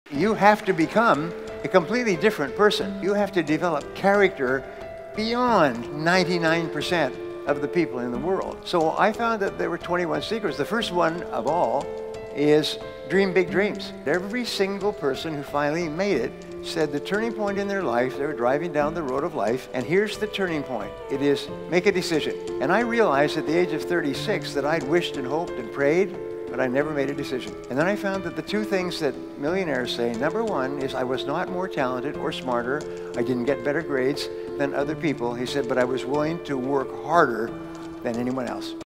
A Millionaire's Mindset on How to Be Successful | Brian Tracy
Brian Tracy talks about millionaires and what they do to become successful.